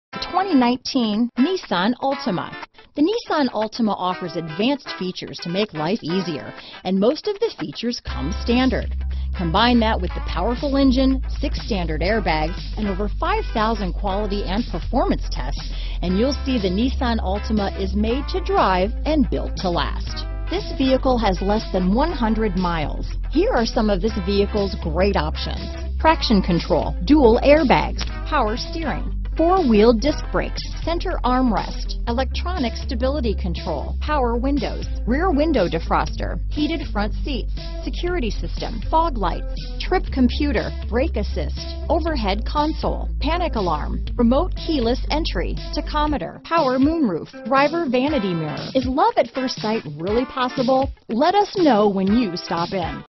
2019 Nissan Altima Pooler, Bluffton, Statesboro, Hinesville, Savannah, GA KC168221
Blue Metallic N 2019 Nissan Altima available in Savannah, Georgia at Vaden Nissan. Servicing the Pooler, Bluffton, Statesboro, Hinesville, GA area.

Vaden Nissan of Savannah
10421 Abercorn